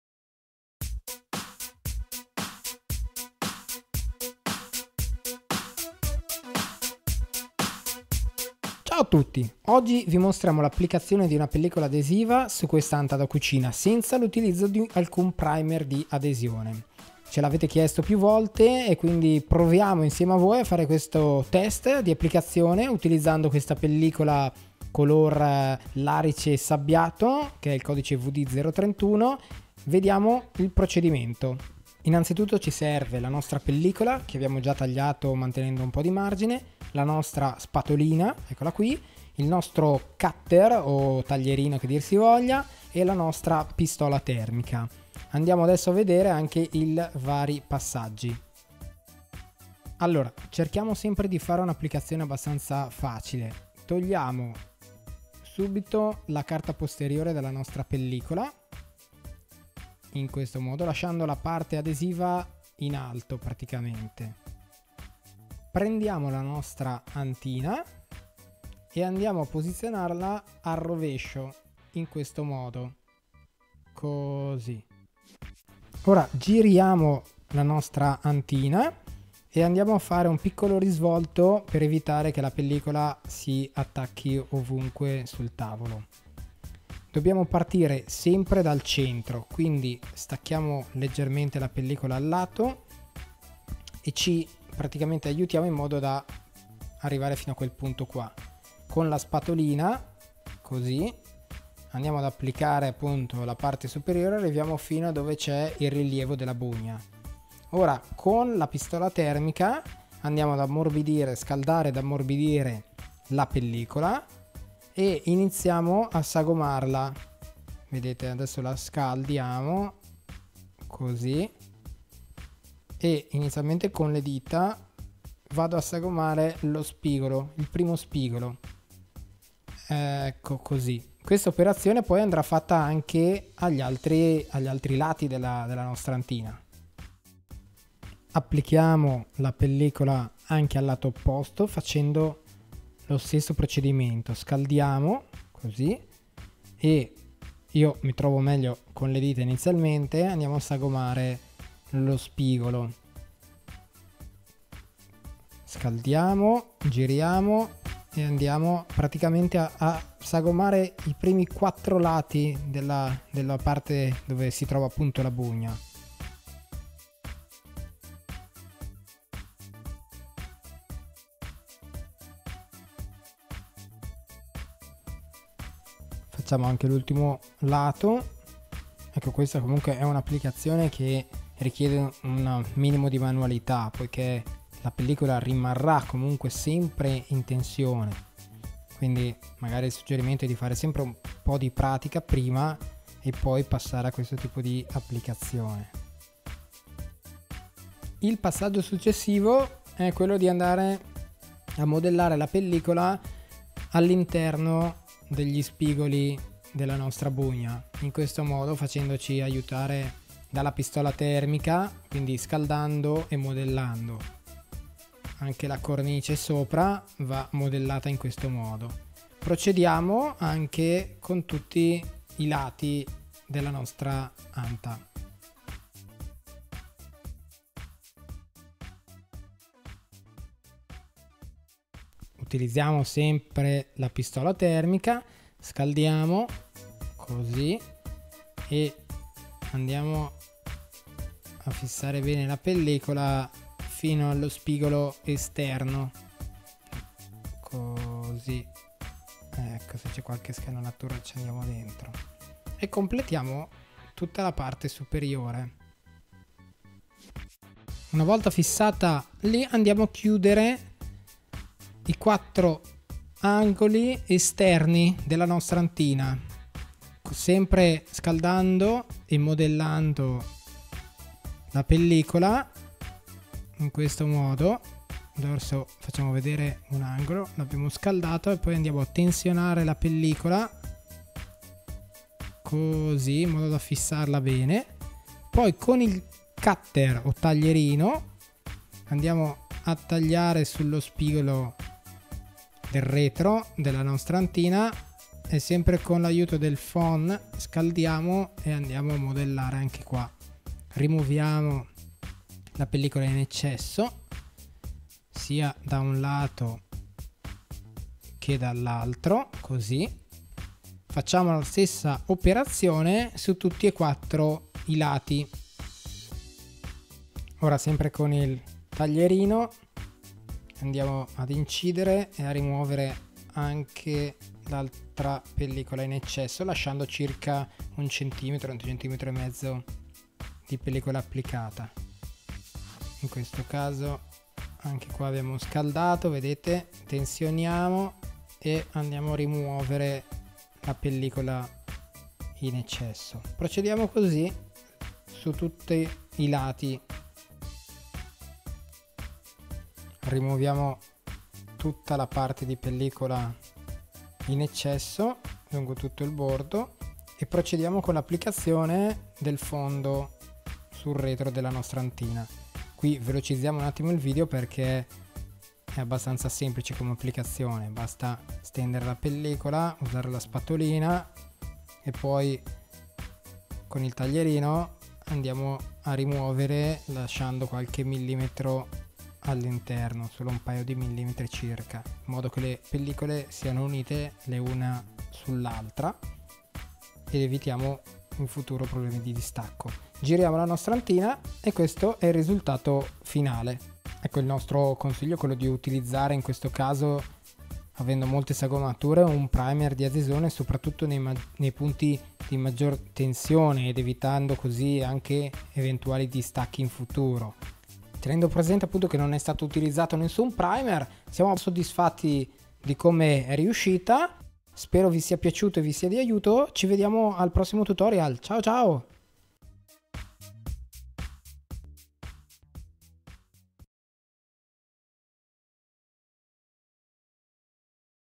Application Vinyl Adhesive Film on Door Kitchen with frame in relief
In this tutorial we show how we applied the self-adhesive film on a kitchen door with embossed frame. It is an application in which a minimum of manual skills is required. in this case no adhesion primer was used.